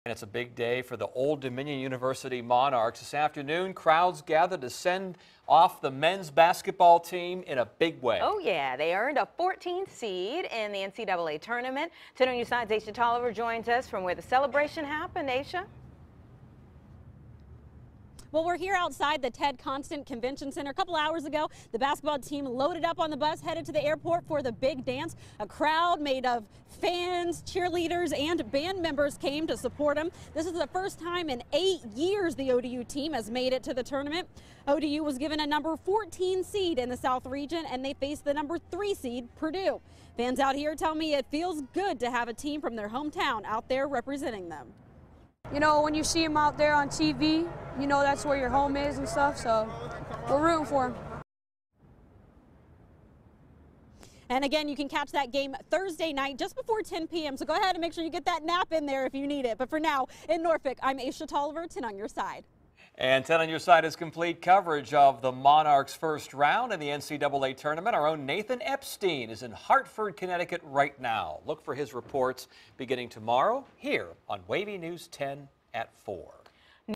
ODU men's basketball departs for NCAA Tournament with send-off in Norfolk
Aesia Toliver reports.